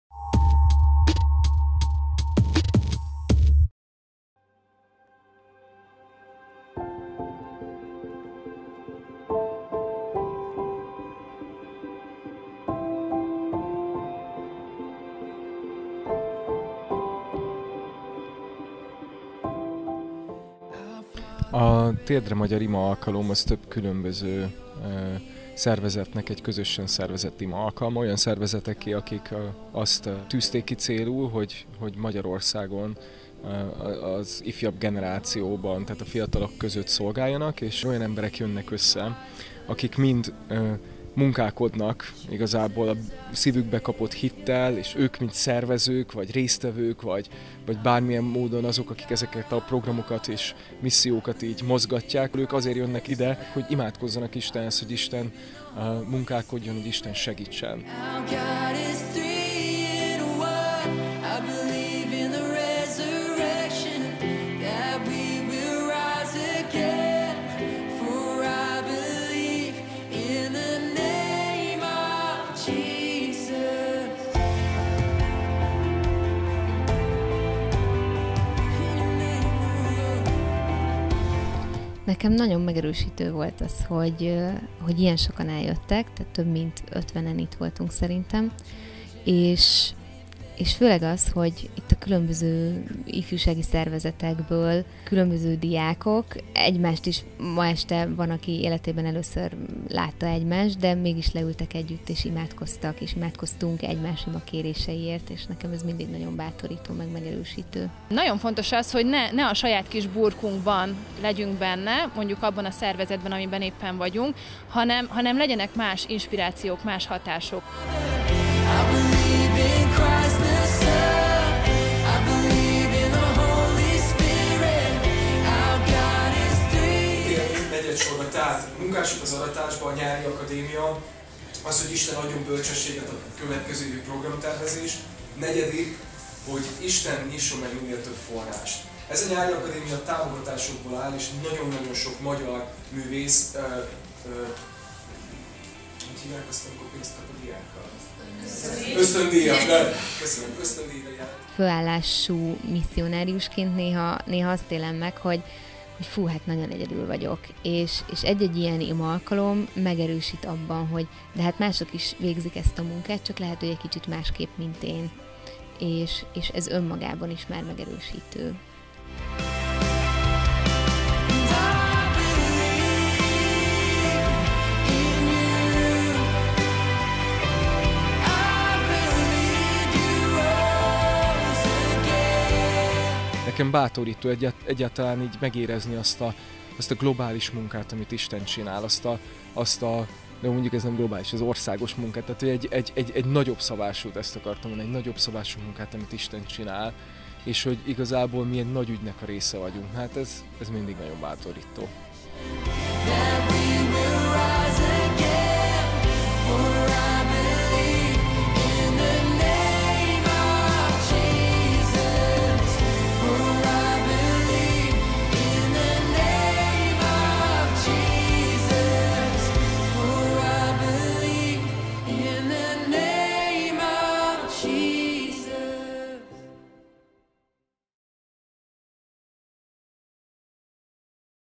Térdre Magyar! - TVSS
MIÉRT VAN A "TÉRDRE MAGYAR"? Missziós Ifjúsági Imamozgalom Budapestért. MEKDSZ :: FÉK :: Budapesti Baptista Ifimisszió - Ott jártunk, forgattunk...